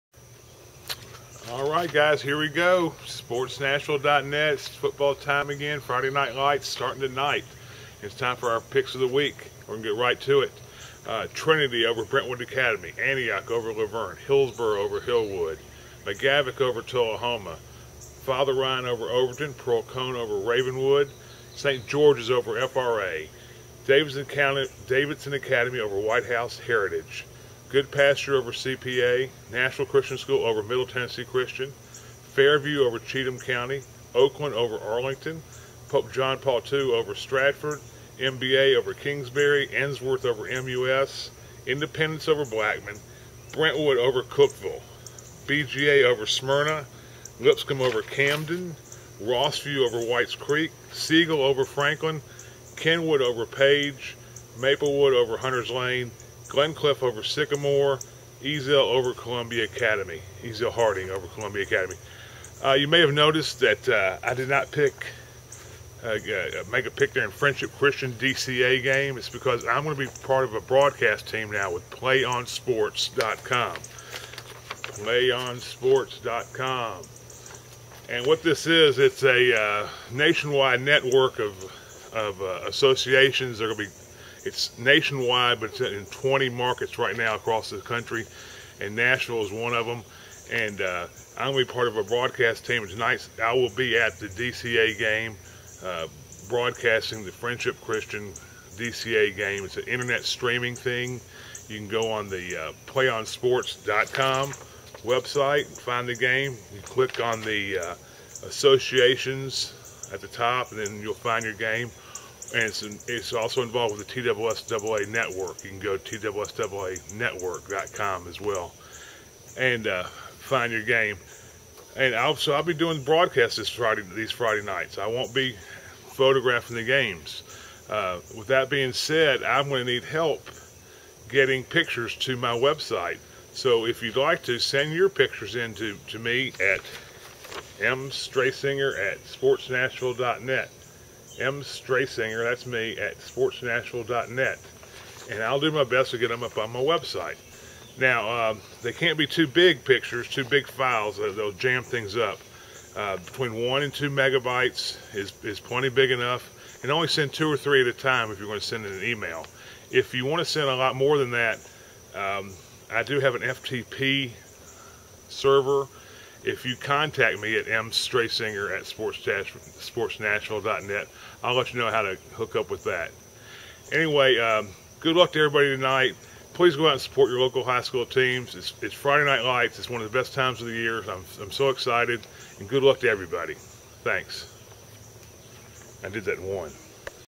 SANY0043
SportsNashville.net's Picks of the Week for Week 0 in High School Football for games in the Nashville Area.